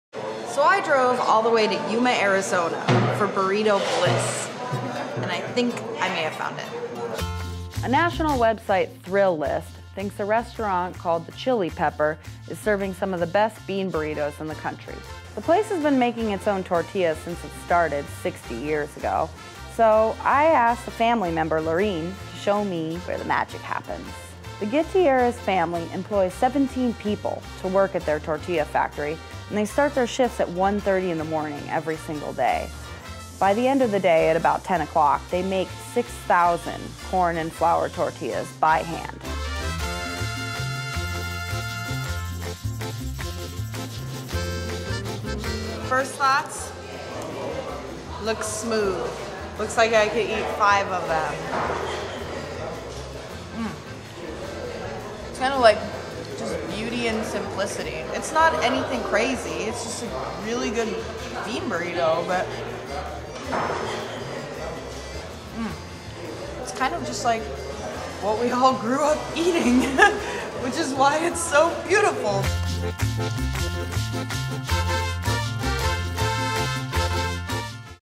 Is America's best burrito in Yuma, Arizona?
The Chile Pepper could be your new stop on the drive to San Diego.